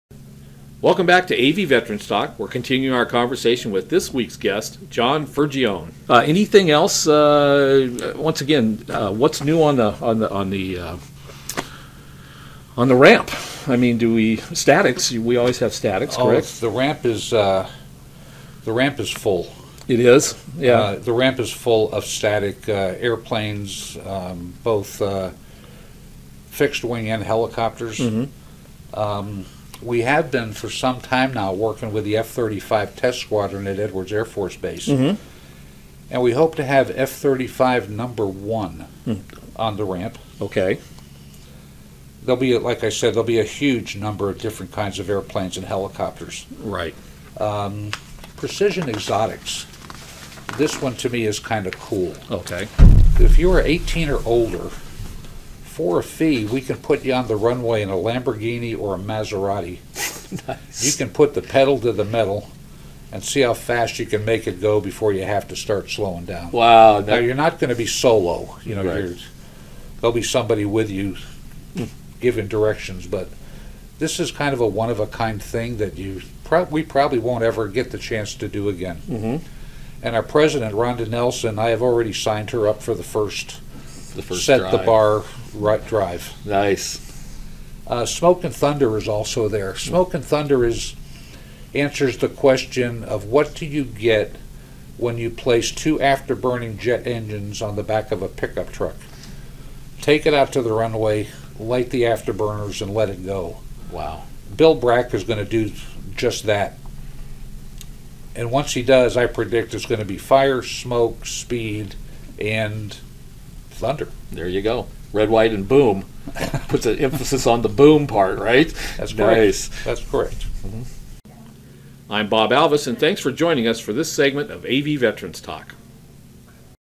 Bob Alvis interviews John Fergione PT2
Bob Alvis welcomes LA County Air Show Executive John Fergione. In this segment, John goes over the lineup of static aircraft displays for this year's air show, scheduled for March 25 & 26 at Fox Airfield in Lancaster, CA. For more information, visit the LA County Air Show